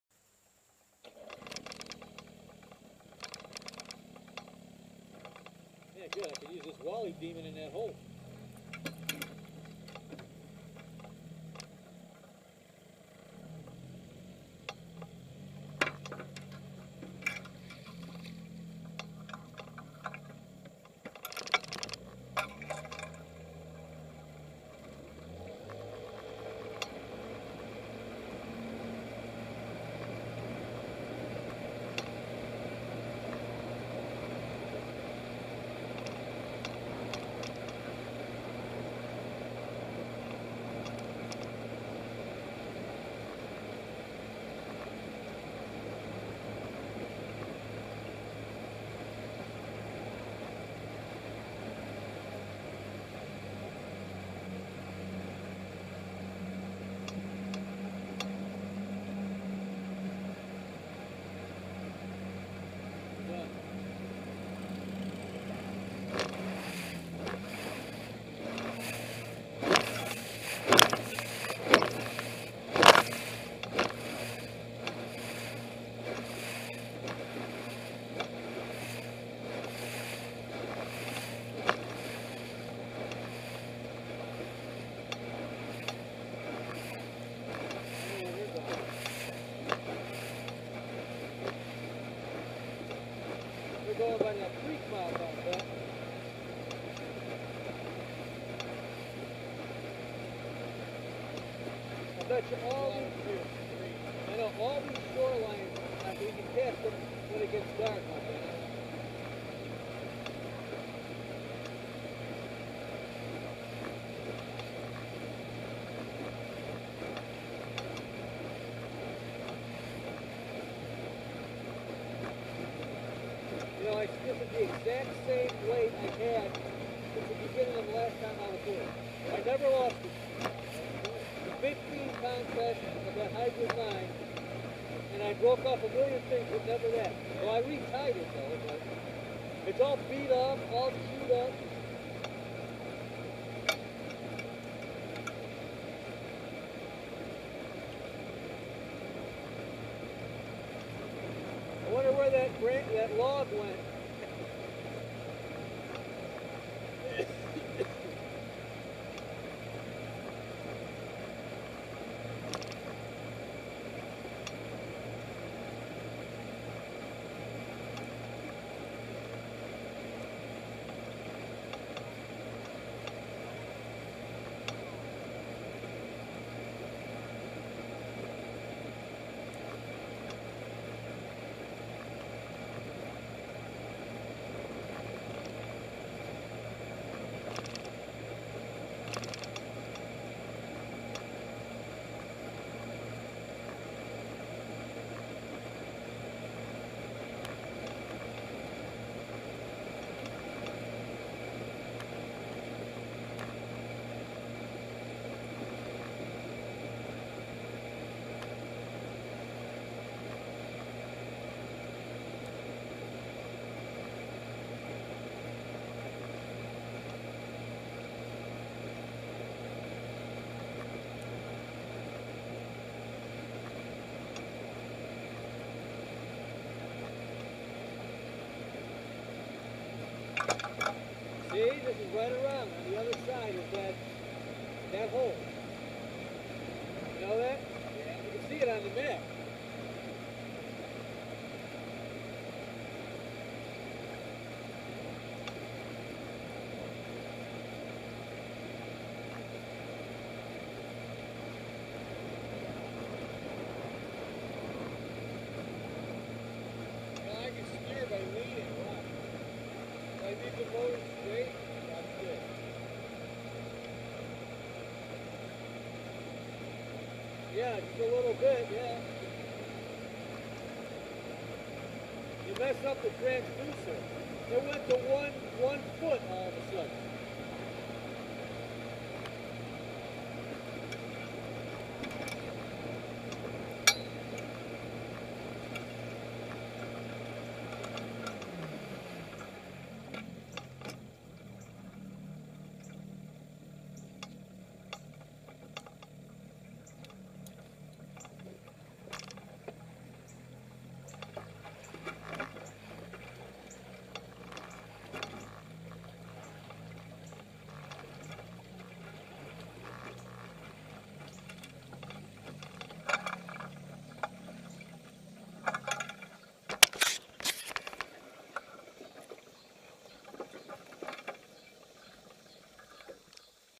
Kankakee River 8/22/14 - 5 min Trip back to a Favorite Spot
Heading back to a particular "spot" on the Kankakee River on a hot day in mid-August.We're in the old Sears Gamefisher jon boat running a 1971 6HP Evinrude that we purchased on ebay earlier this year. The motor was well maintained by the previous owner and still runs great. He really hated to part with it.

The River was rising and fishing was tough.